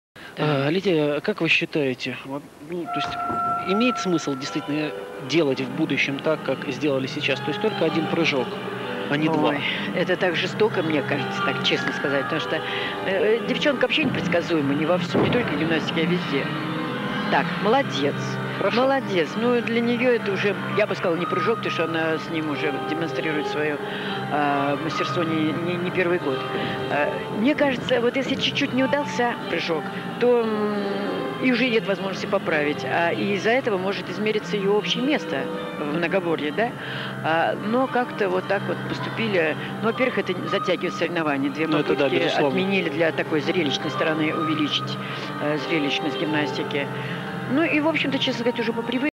Natalia ZIGANSHINA vault - 2005 Russian nationals AA
Natalia ZIGANSHINA / Наталья Зиганшина

'05 Russian nationals All Around / Чемпионат России Индивидуальное многоборье

8.850
Rank : 6th